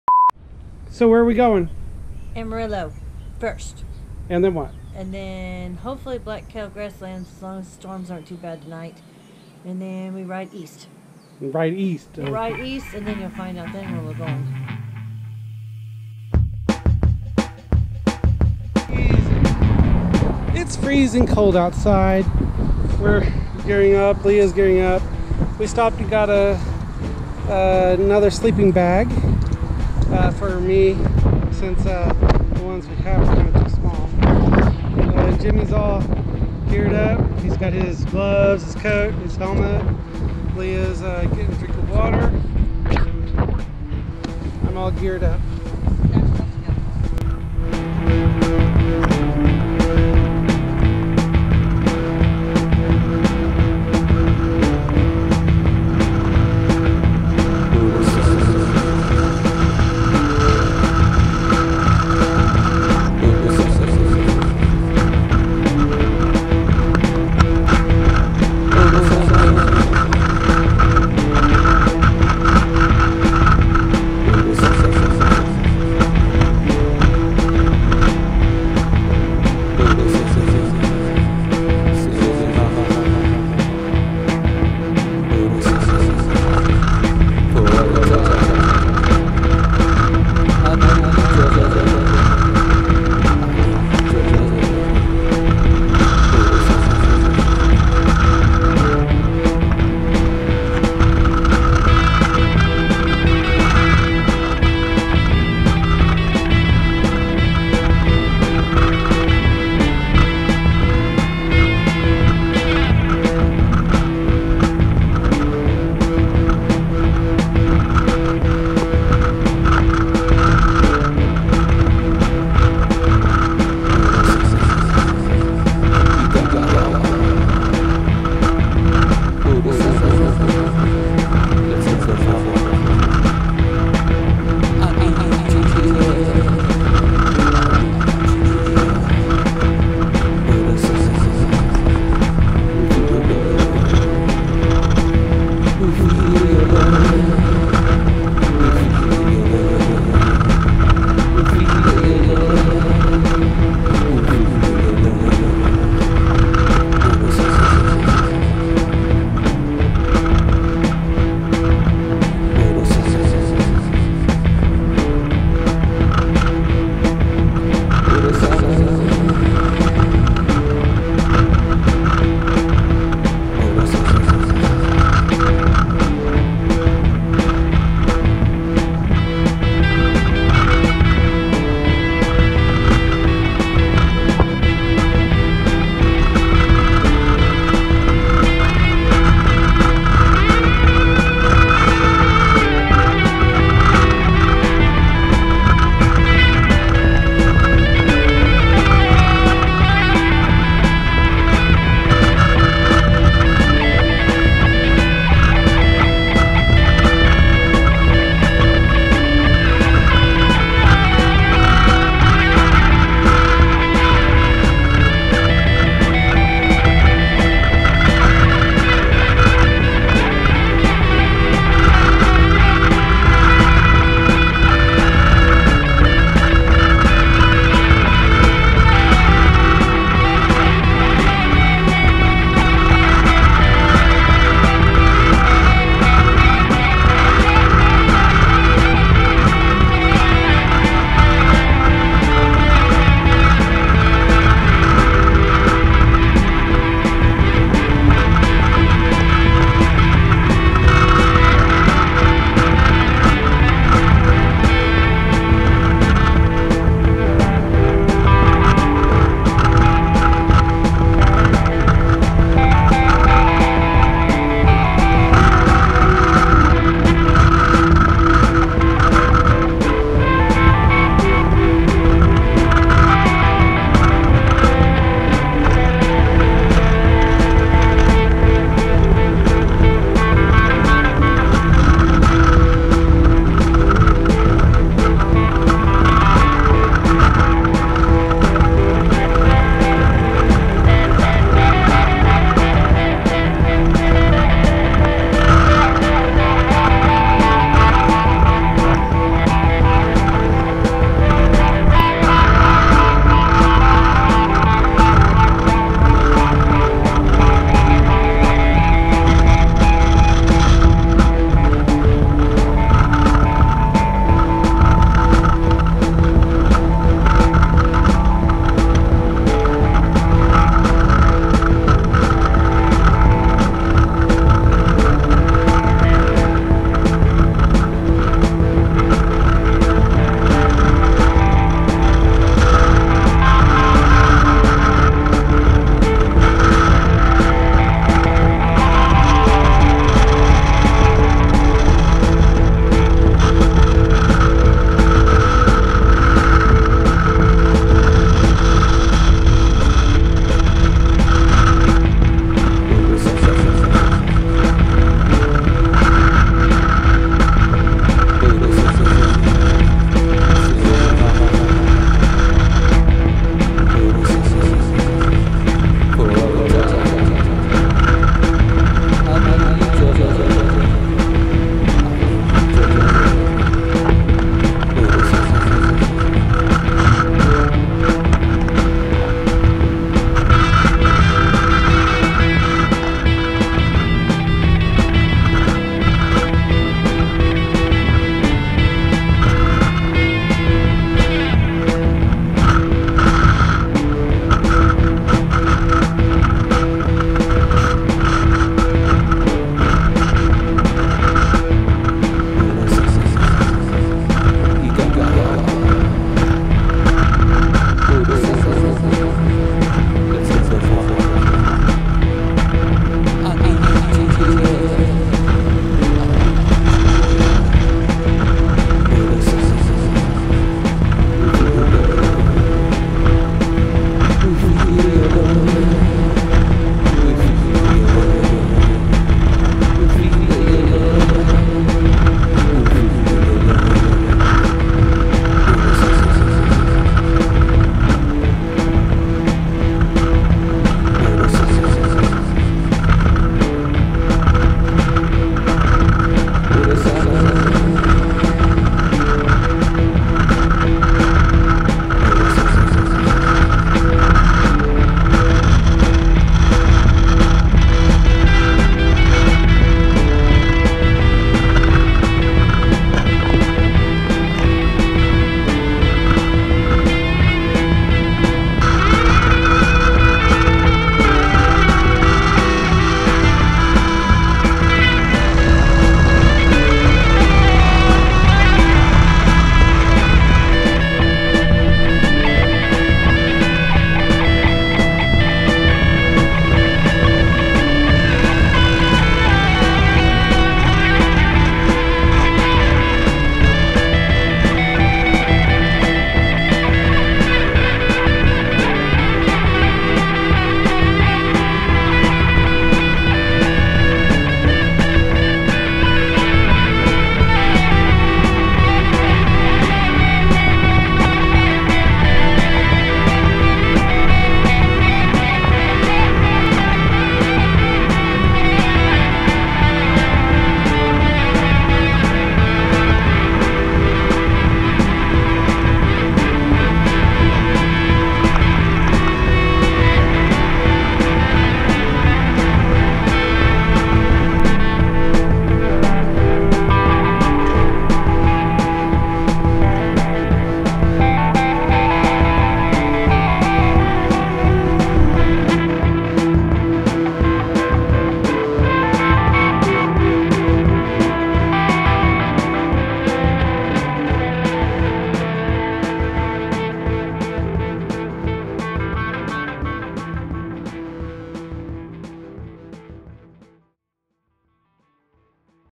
Epic Motorcycle Quarantine Trip Day 2
During the Great 2020 Quarantine, we decided to take a trip on our Motorcycle. It was all planned out. However, we quickly found out that we would need to change our plans several times.

Day two We woke up to a low of 42 degrees without windchill. Got some warmer gear to put on under our outerwear and hit the road and head South from Amarillo, TX to see the Palo Duro Canyon. Instead of going into the state park, we found a Texas State Highway that when right through the canyon. After that, we head back into Oklahoma to Spring Creek Lake Campground.

Drytech Bag

JetBoil Camping Stove

ALPS Mountaineering Taurus 4-Person Tent

ROK Straps ROK-10050 Black/Reflective 18" - 60" Motorcycle/ATV Adjustable Stretch Strap